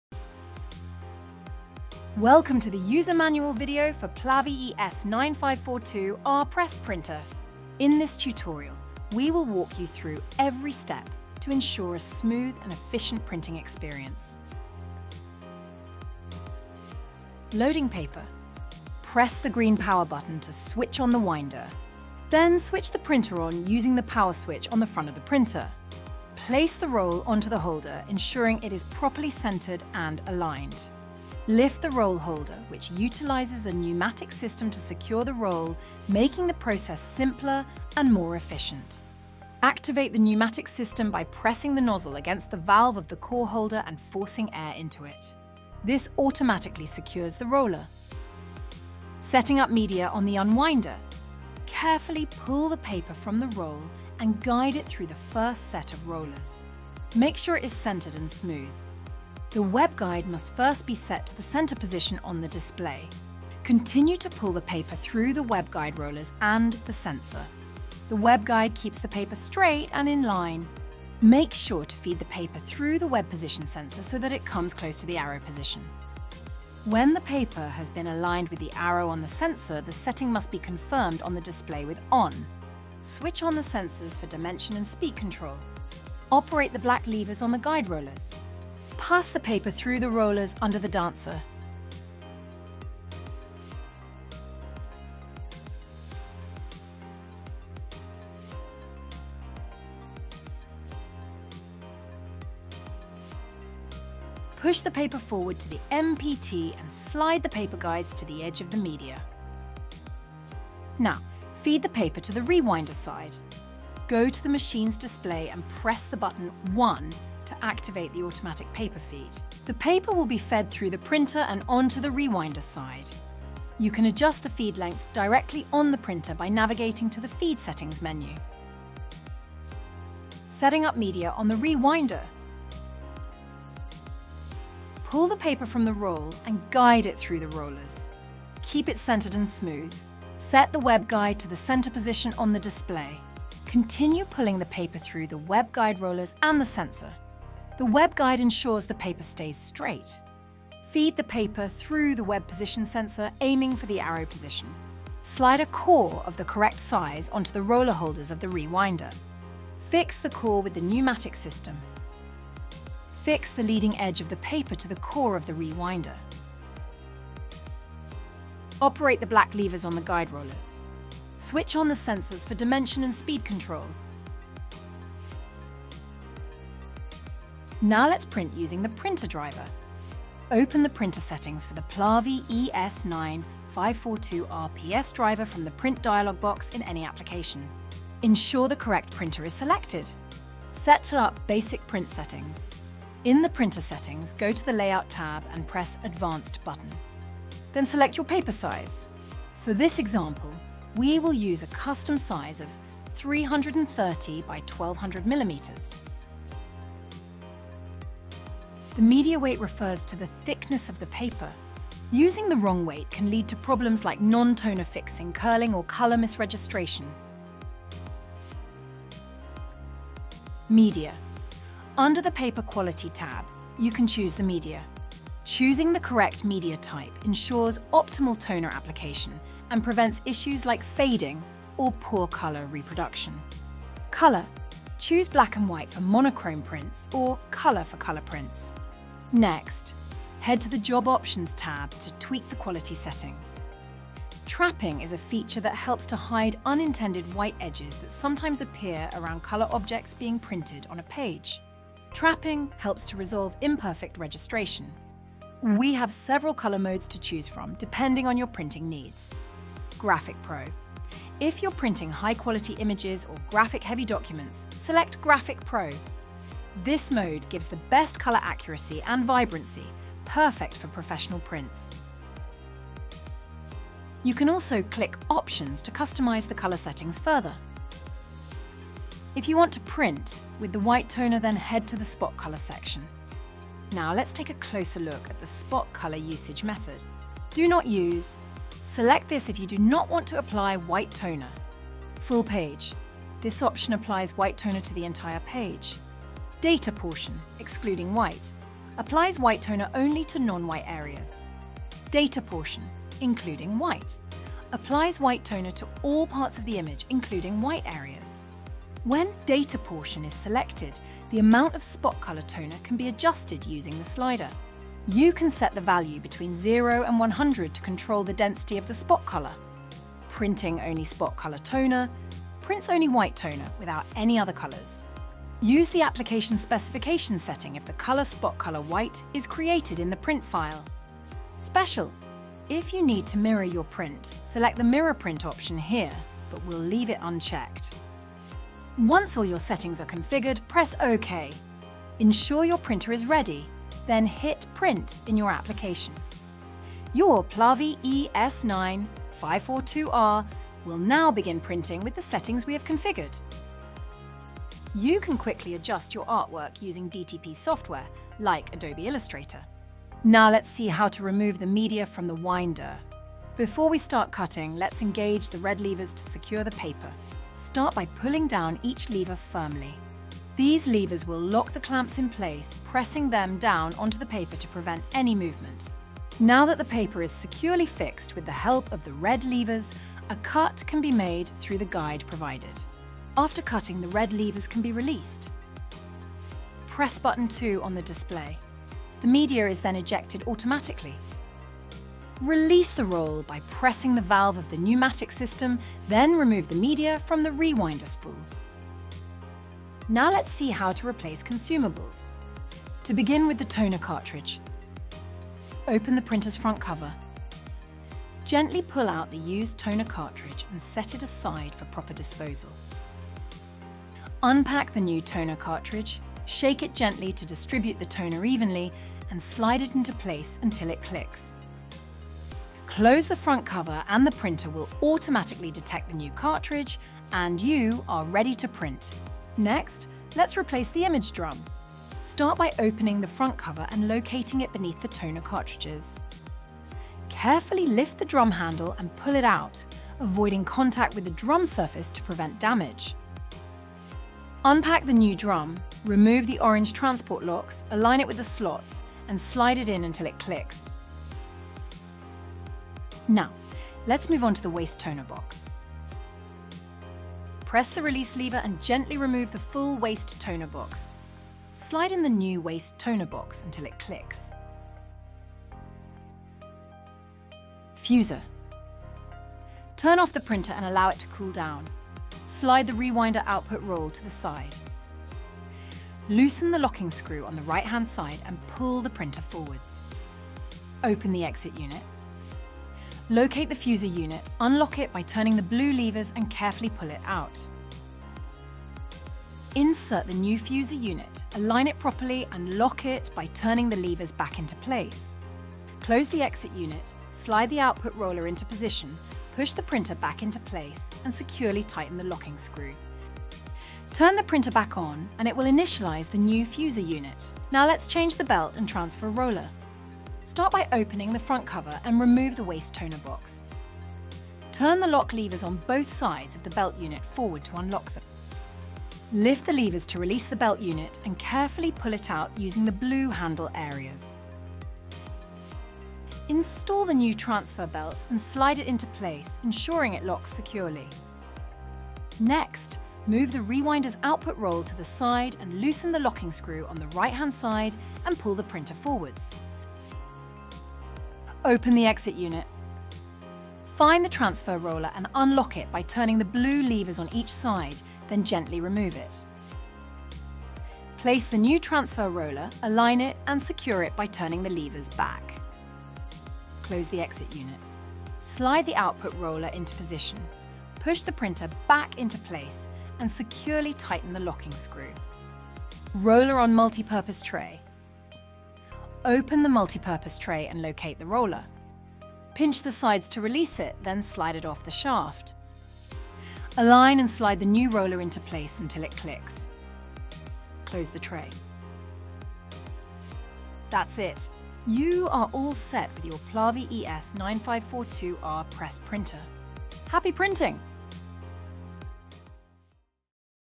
OKI PLAVI ES9542R: User Manual - Step-by-Step Guide for Wide-Format Digital Label Press Printer
Get the most out of your OKI PLAVI ES9542R with this comprehensive tutorial video. Learn how to operate this 5-colour label press (CMYK + WHITE toner) for waterproof, alcohol-resistant, and grease-proof labels. OKI’s A3+ roll-to-roll digital printer handles media up to 330 x 1,320 mm and continuous runs up to 200 meters, making it perfect for short and high-volume production. Ideal for creating durable labels for outdoor, retail, and cold storage applications.

Contents:
1. Loading paper
2. Setting up media on the unwinder
3. Feeding the media into the printer
4. Setting up the media on the rewinder
5. Print with the printer driver / PS Driver
6. Change artworks to print different media
7. How to replace consumables